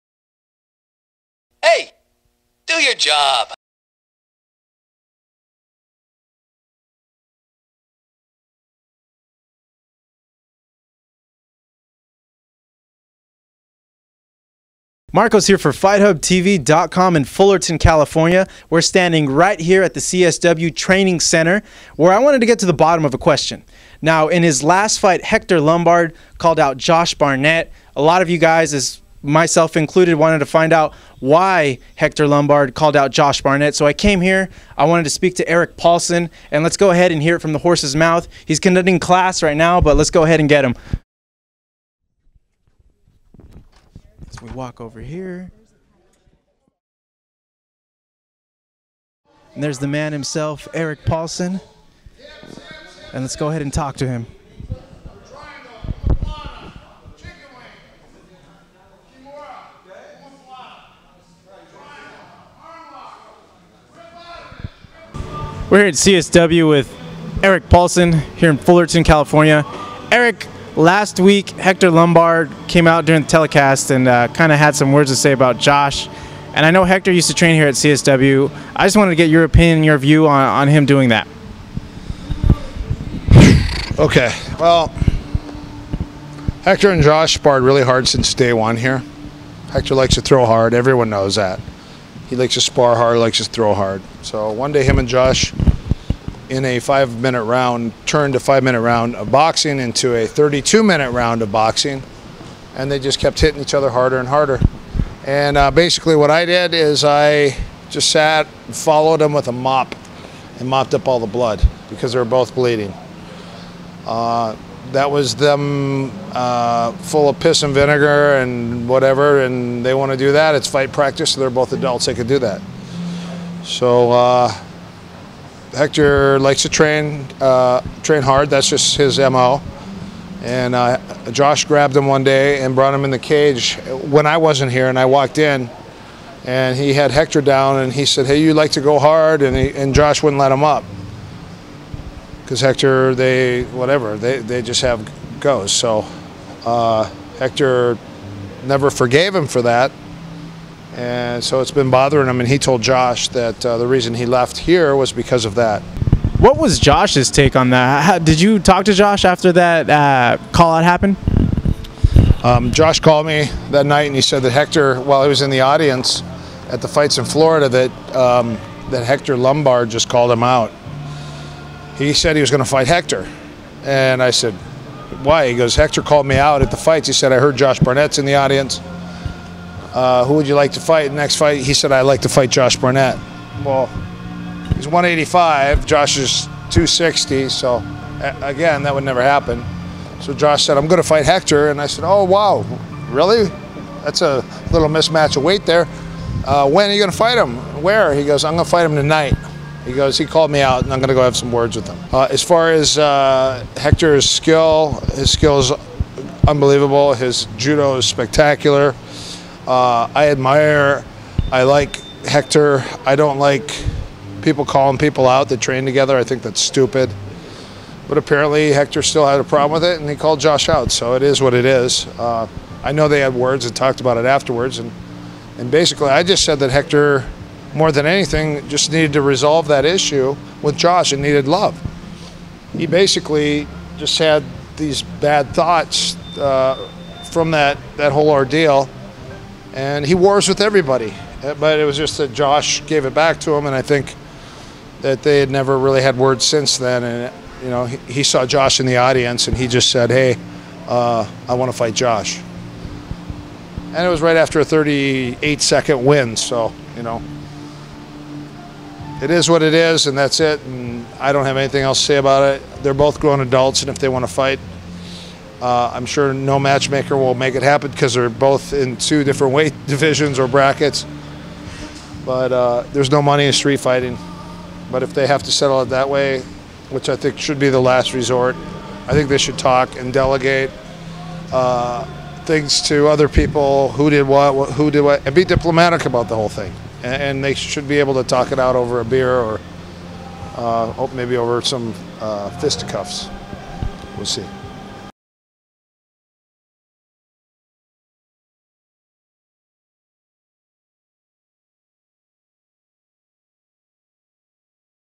Erik Paulson reveals why there is beef between Josh Barnett and Hector Lombard
Fight Hub TV sent it 's fight correspondent Marcos, on a mission to dig up why there is beef between Josh Barnett and Hector Lombard. He went straight to the source as he visited CSW in fullerton, former training ground of Lombard and current training center of Josh Barnett to get the scoop on the story between these two.